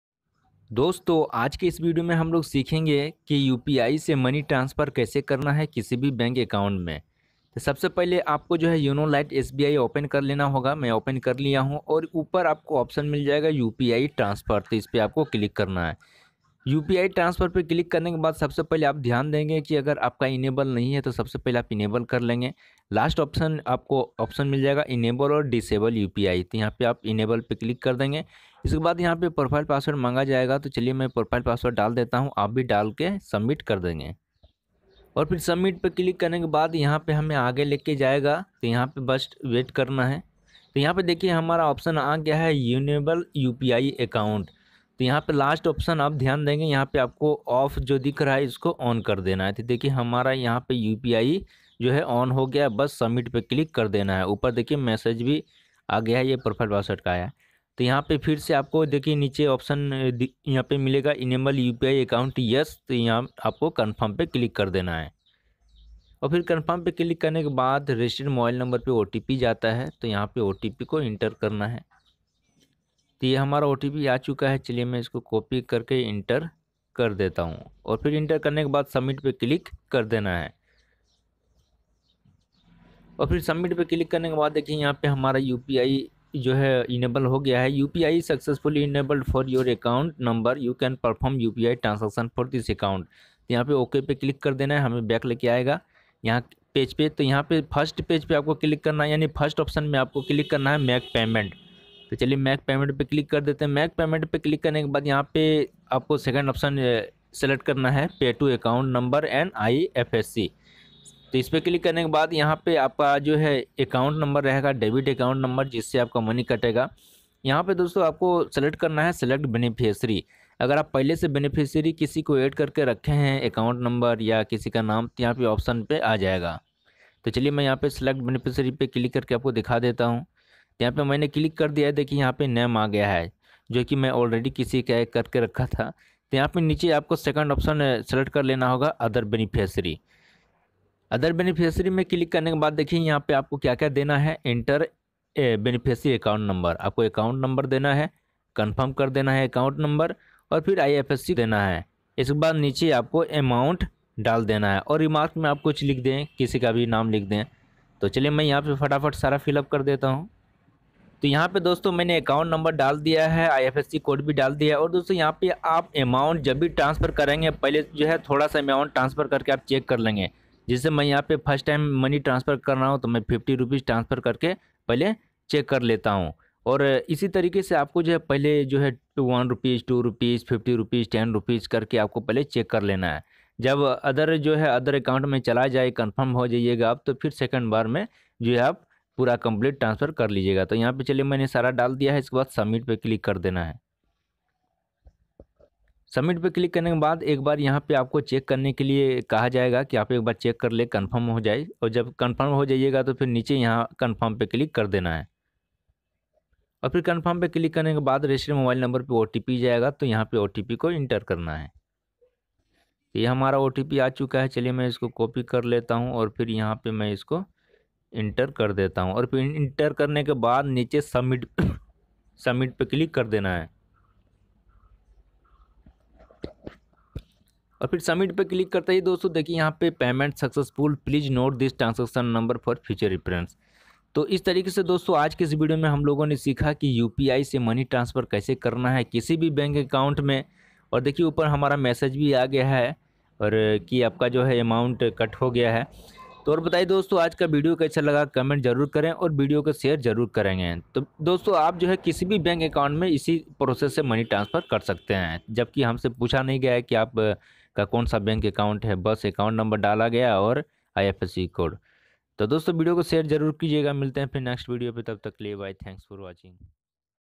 Yono lite sbi se paise kaise transfer kare | How to send money from yono lite app to another account
My gears